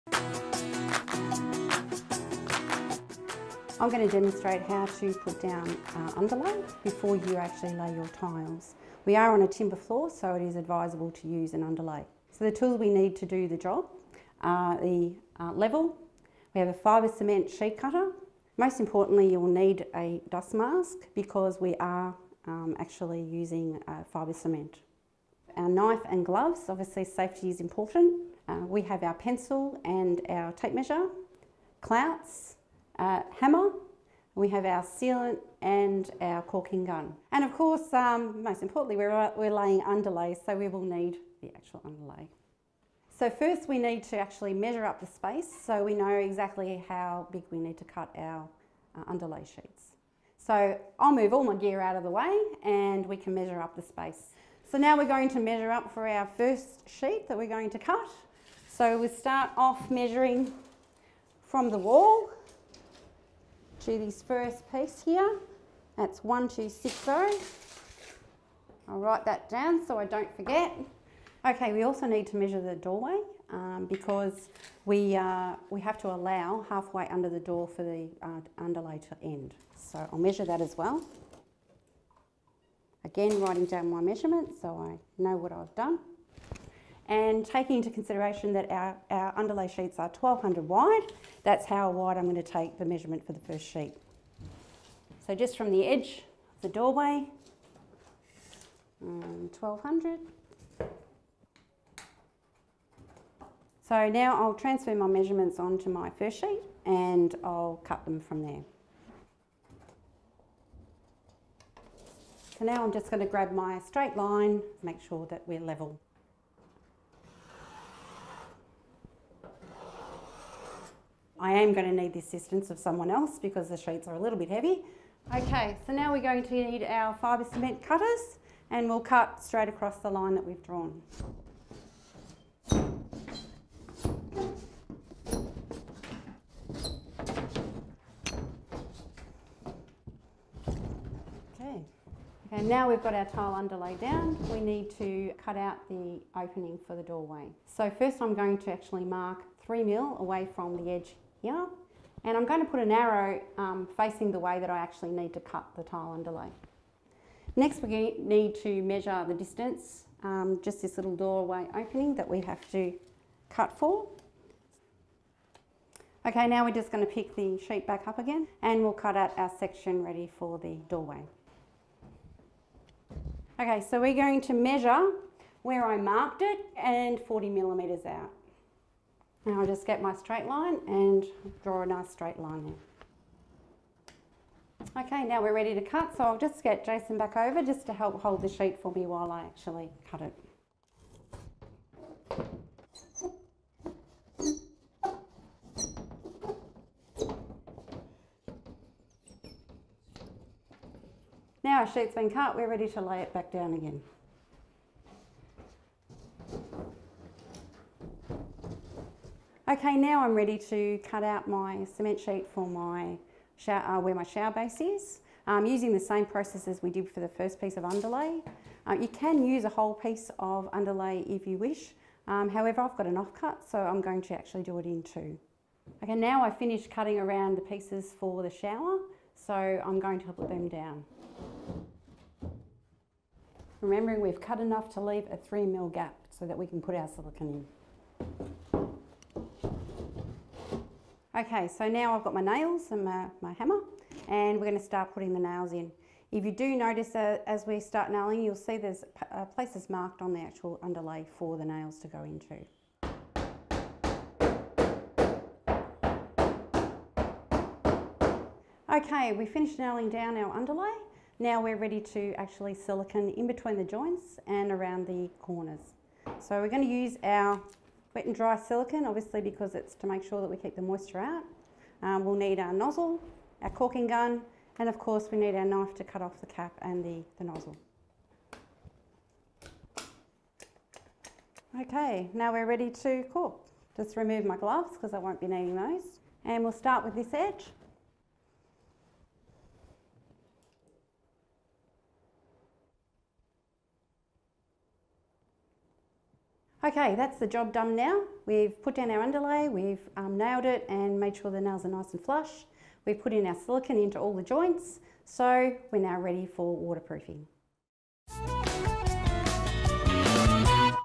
How To Install Tile Underlay - DIY At Bunnings
When you need to lay floor tiles in your home, it’s important to create an even, durable surface. This video from Bunnings Warehouse will show you how easy it is to install tile underlay.